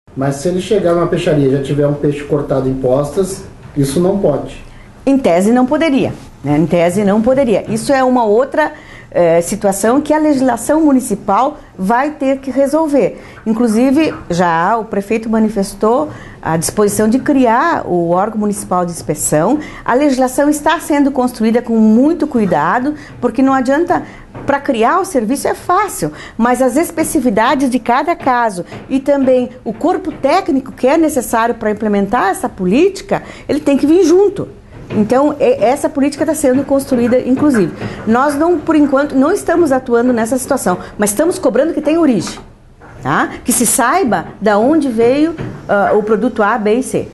A peixaria sem entreposto pode vender peixe em posta?
Para um peixe ser vendido em partes, ou postas, ele deve ter sido processado num entreposto. A peixaria que não tem entreposto já deve vender as partes processadas. Mas questões culturais impedem a plena aplicação da lei. Entenda a questão com a  promotora Sonia Maria Piardi.